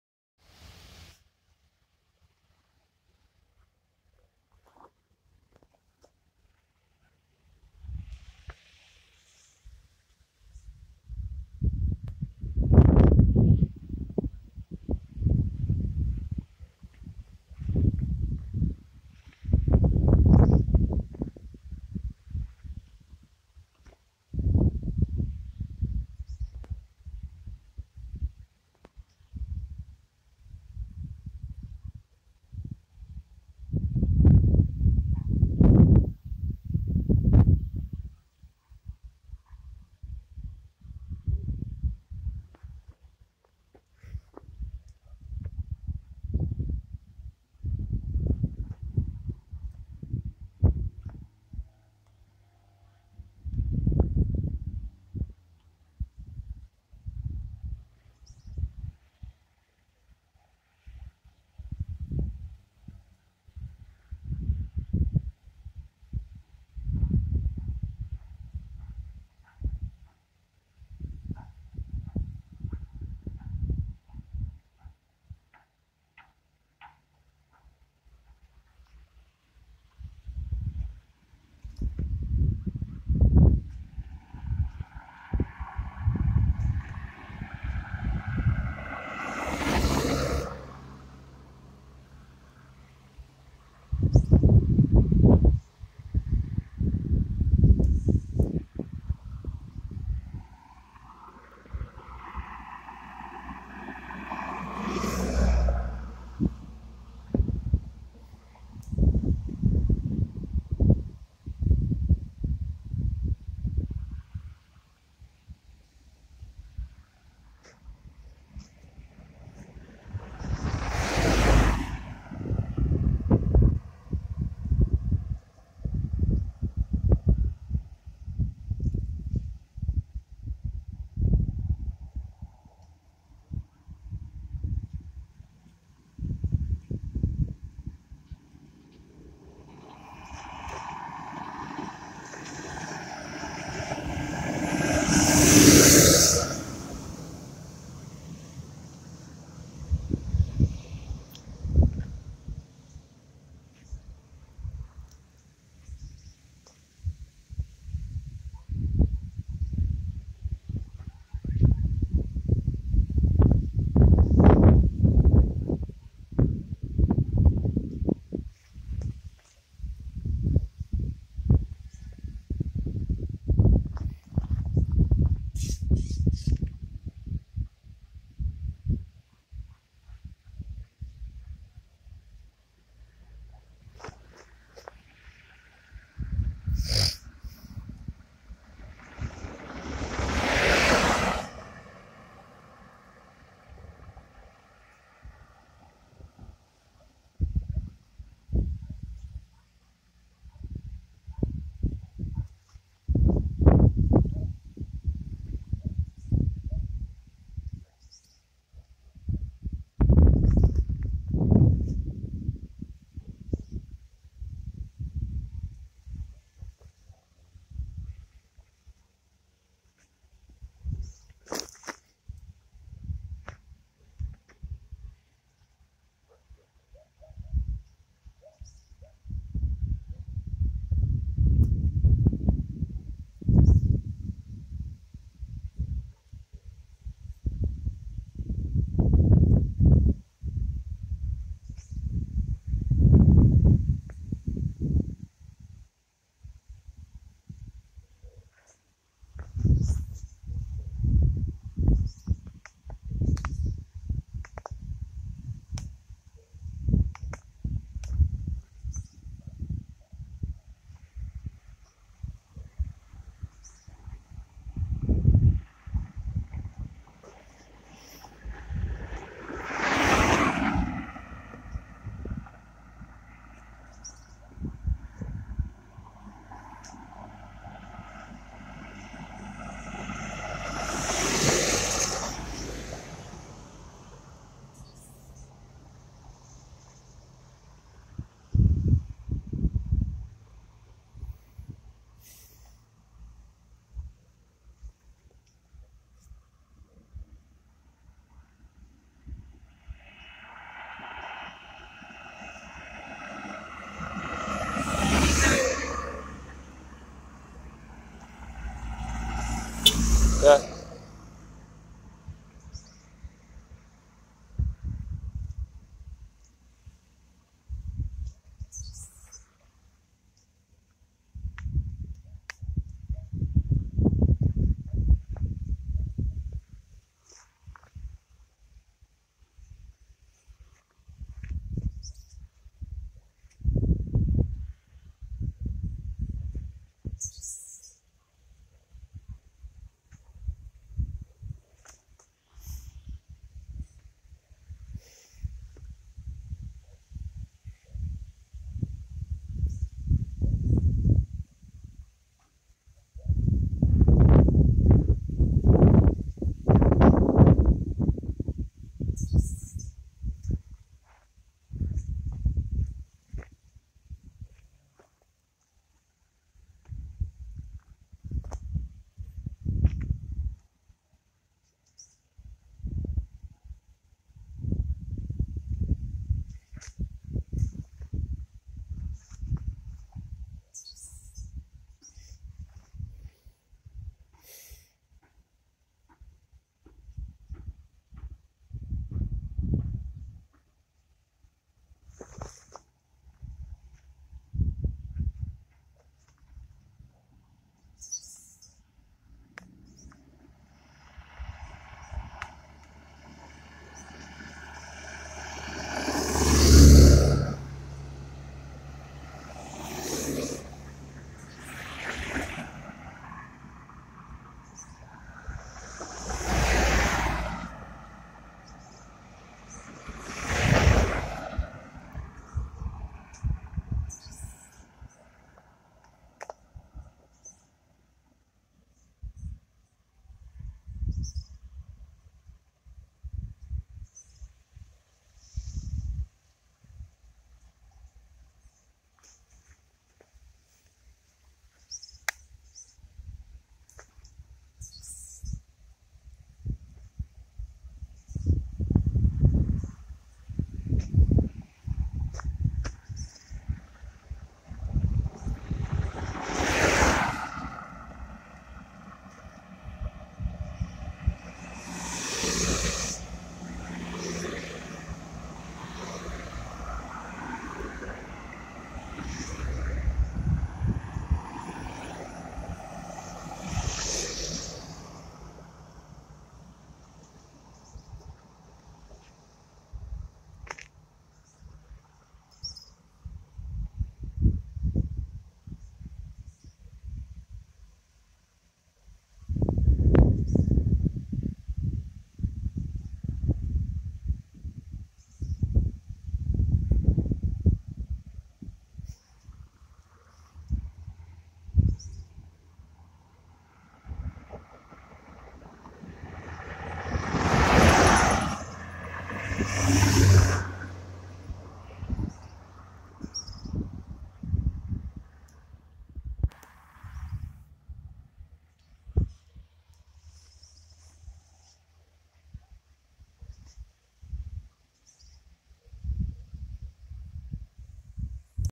Locust Hall St. George Barbados 🇧🇧 Part 3
This is the section of Locust Hall Hall heading towards Applewhaites